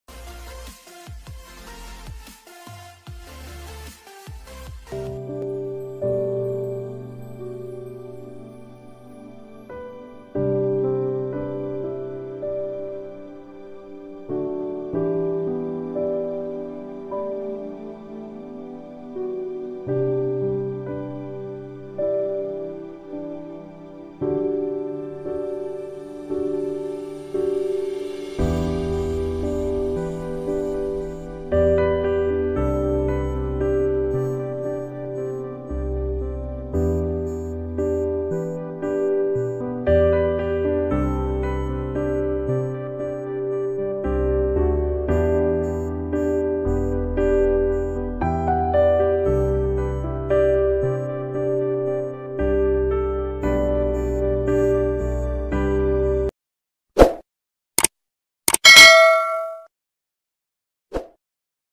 Arabic calligraphy Allahu Akbar | Arabic calligraphy art | Islamic art(الله أكبر)
Today I will be art Arabic Calligraphy Allahu Akbar. It's a Very Easy Art tutorial.